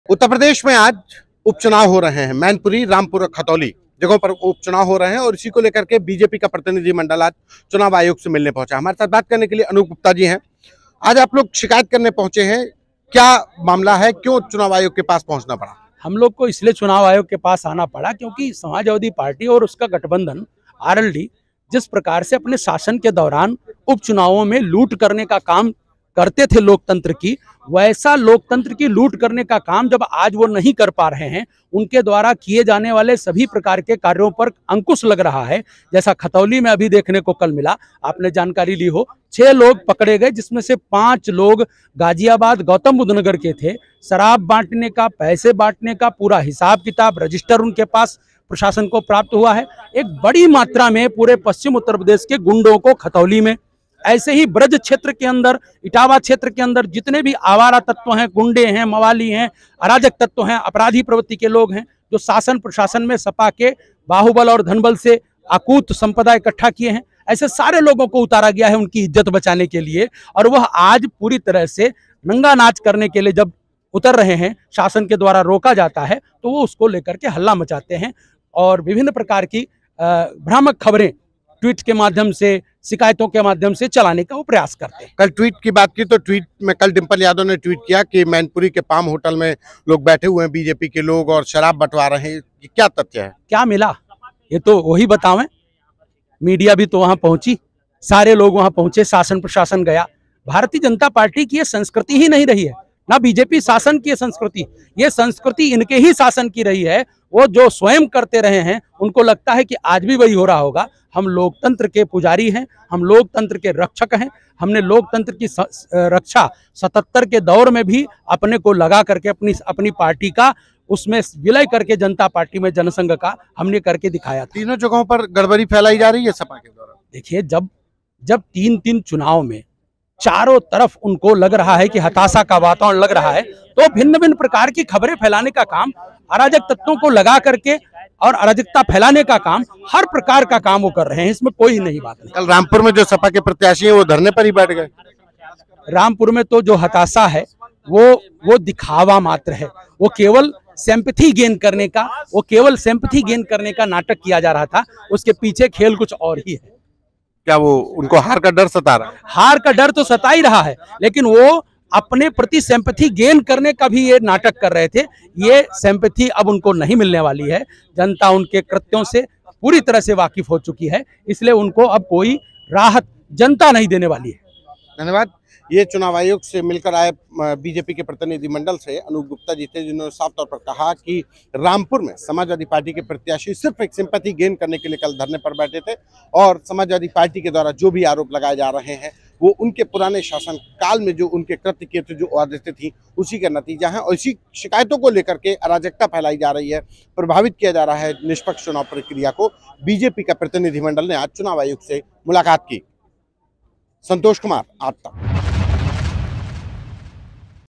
UP By-Elections: चुनाव आयोग पहुंचा BJP का प्रतिनिधिमंडल, जानिए क्या है पूरा मामला | AajTak
उत्तर प्रदेश में आज 3 सीटों पर उपचुनाव हो रहे हैं. एक तरफ समाजवादी पार्टी है जो योगी सरकार और प्रशासन पर आरोप लगा रही है कि वो सपा के समर्थकों को वोट नहीं डालने दे रहे. जबकि दूसरी तरफ बीजेपी है जिसका एक प्रतिनिधिमंडल चुनाव आयोग में शिकायत लेकर पहुंचा. जानें क्या है पूरा मामला.

By-elections are being held on 3 seats in Uttar Pradesh today. On one side there is the Samajwadi Party which is accusing the Yogi government and the administration of not allowing SP supporters to vote. While on the other side is the BJP, whose delegation reached the Election Commission with a complaint. Know what is the whole matter.